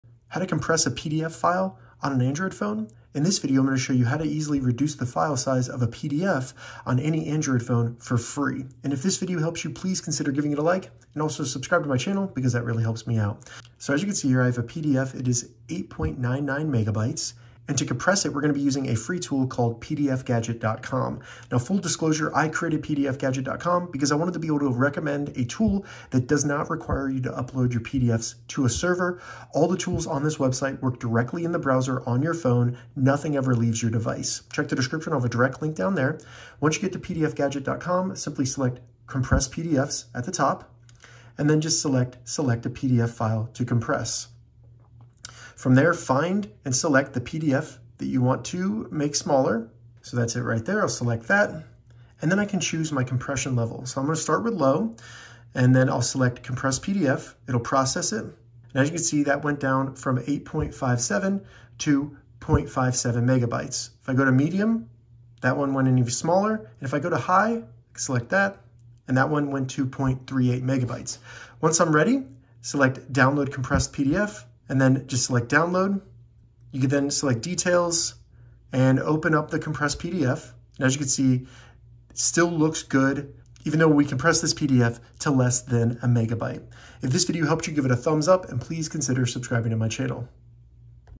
How to Compress PDF Files on Android
How to Compress PDF Files on a Android Phone for Free? Easily reduce the file size of your PDF documents directly from your Android device without needing a computer or app. This means you can quickly shrink large PDFs to save space, upload faster, and make sharing easier completely free and with no sign-up required. To do this we use a tool I built called PDFGadget. PDFGadget allows you to compress PDFs for free without installing software or uploading files to servers because it all works in the browser on your Android phone.